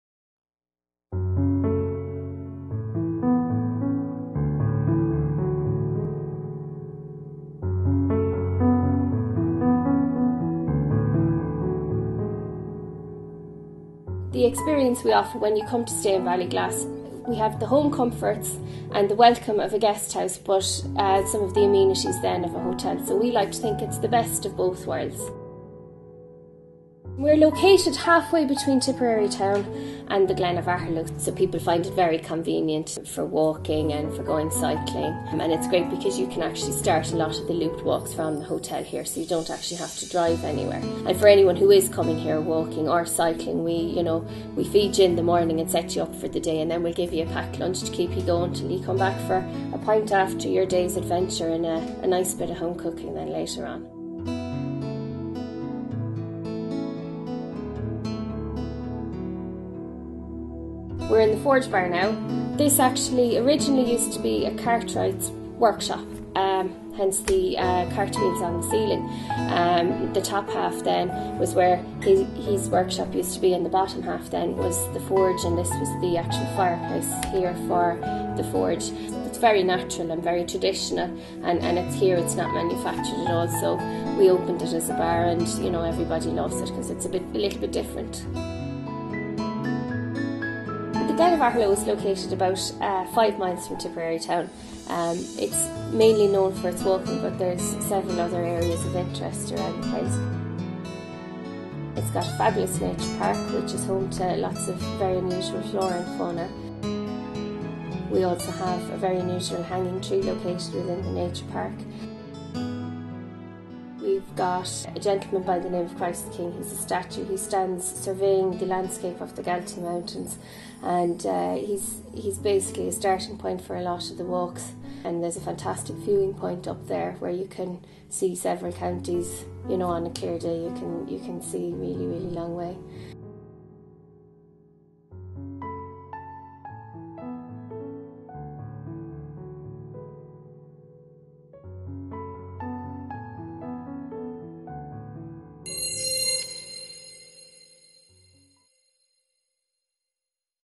Glen of Aherlow & BallyGlass House Hotel
Ruth welcomes us to Ballyglass House Hotel